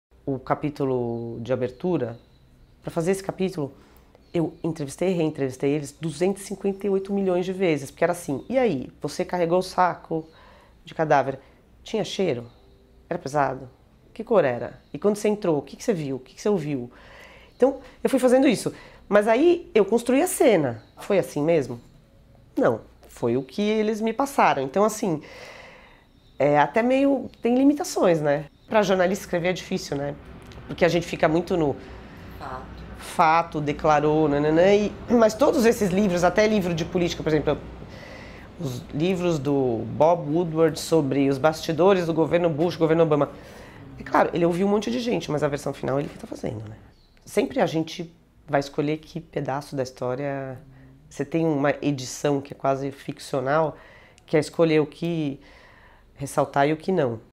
Trecho da entrevista com Patrícia Campos Mello, autora de Lua de mel em Kobane
A repórter especial e colunista da Folha de São Paulo Patricia Campos Mello lançou em 2017, Lua de mel em Kobane (Companhia das Letras), um livro de não-ficção sobre uma história de amor em plena guerra da Síria. Neste trecho, a autora fala sobre as limitações da ficcionalização do real. A entrevista na íntegra sai sexta-feira que vem!